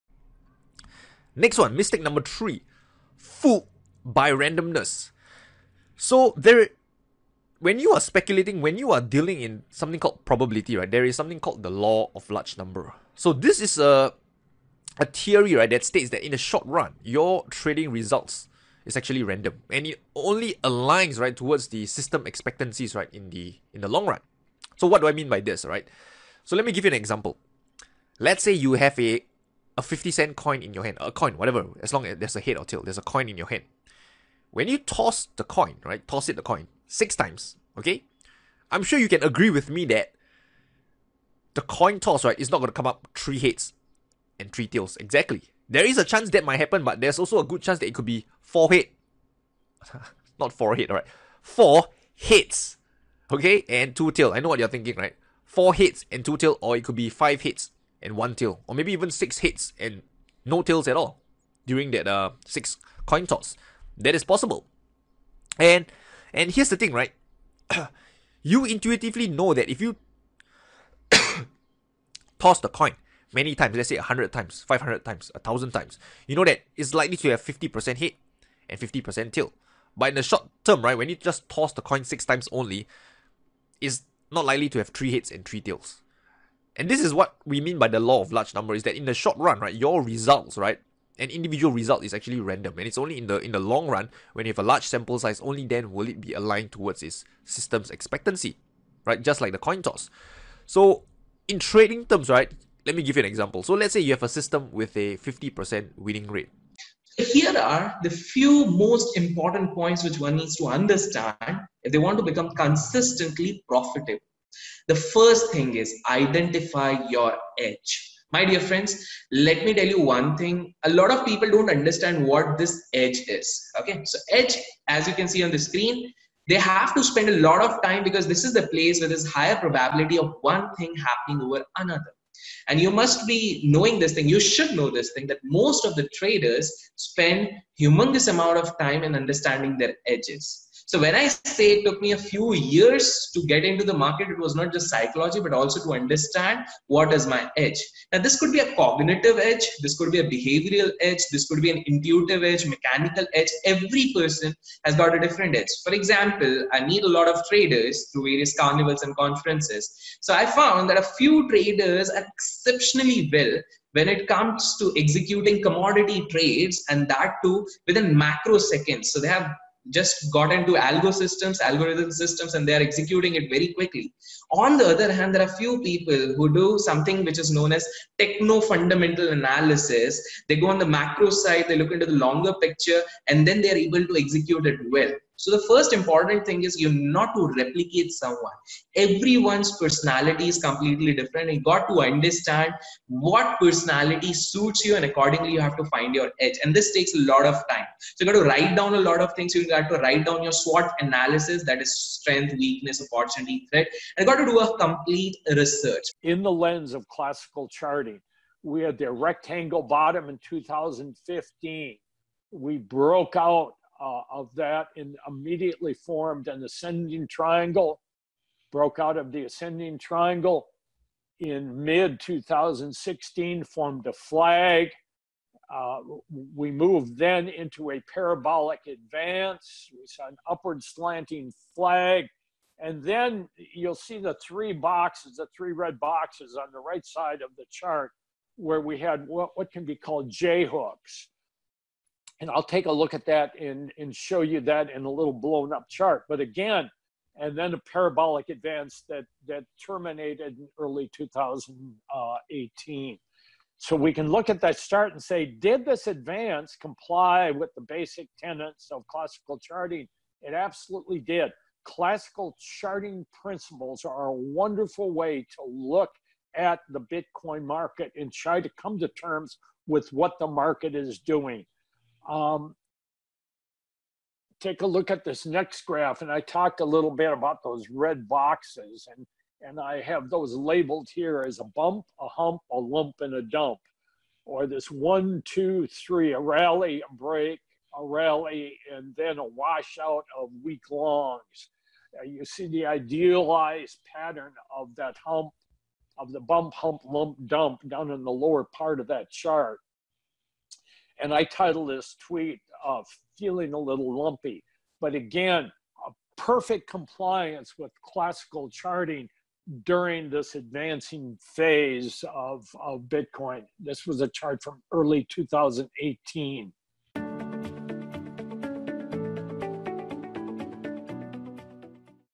OTS 2019 - Day 1 Trailer - Rayner Teo, Abhishek Kar​ and Peter Brandt​
Want to hear from top traders like Rayner Teo, Abhishek Kar​ and Peter Brandt​ share their personal trading secrets with you today?

Here are the topics they are covering today:
- Trading Principles: Profitable Trading Lessons From A Decade Of Trading
- Trading Psychology: The Essential Steps To Improving Your Mental Edge
- Crypto Trading: Perspectives From A Career Commodity Trader​

Join us and over 25k+ traders today at Online Trading Summit 2019